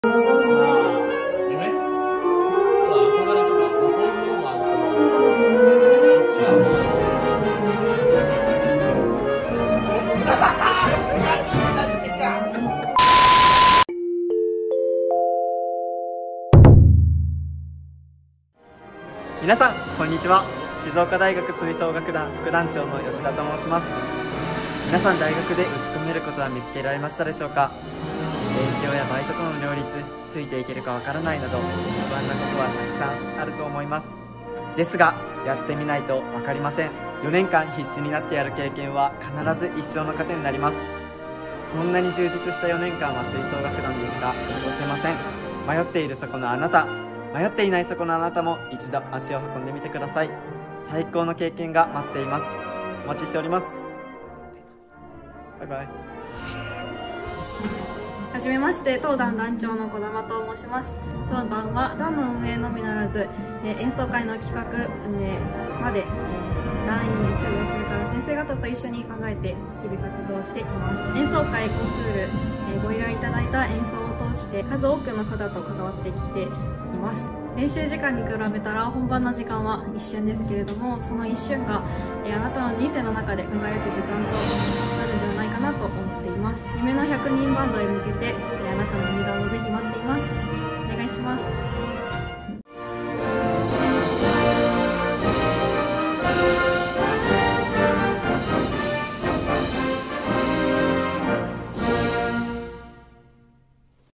【来たれ！新入生】副団長と団長(とトロンボーン吹き)からのメッセージ
静岡大学吹奏楽団では、音楽を楽しみたい大学生の入団を心よりお待ちしております！
全力で、輝く大学生活を一緒に送りましょう！！